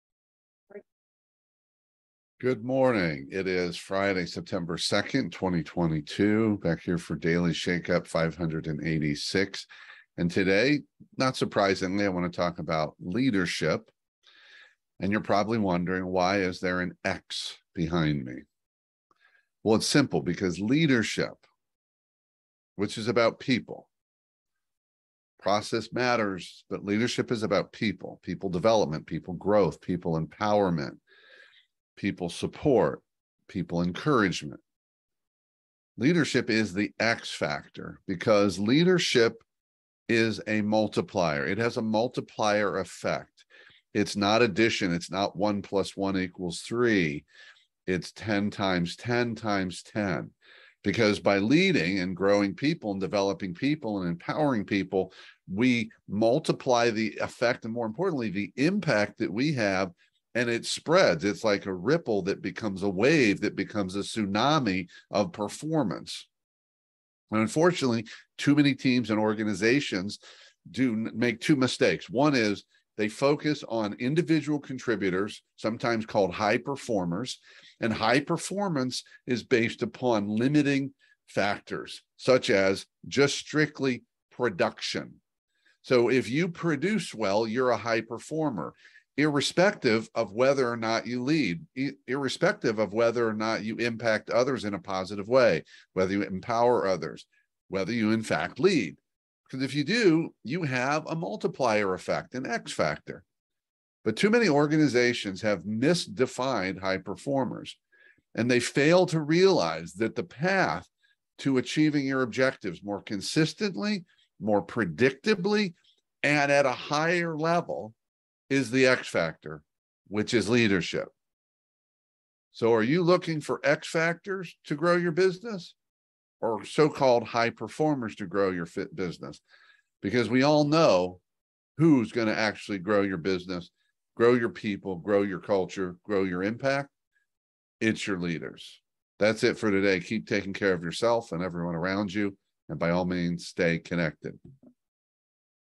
Daily Shake Up 586 - Leaders Are Multipliers
Which would you like to have more of in your organization?

High performers or empowering leaders?

If you say both, good answer but often we have to choose between the two, especially when we make the mistake of assessing performance based upon individual production (almost to the exclusion of leadership).

Daily Shake Up 586 is a reminder the leadership is the “X” factor – the multiplier effect – in any business and with any team. Leaders empower, develop, engage, inspire, grow, support and amplify people and their collective performance – thus, the “X” factor.

Leaders create ripples that become waves that become tsunamis of performance, execution, results and impact for the entire team and organization, while high performers typically only create their own waves. While a high performer’s wave may bigger than everyone else’s wave, it’s small when compared to the “X” factor that results from leadership.

If you want to “X” your business, focus on building and developing your leaders, and you can never have too many leaders.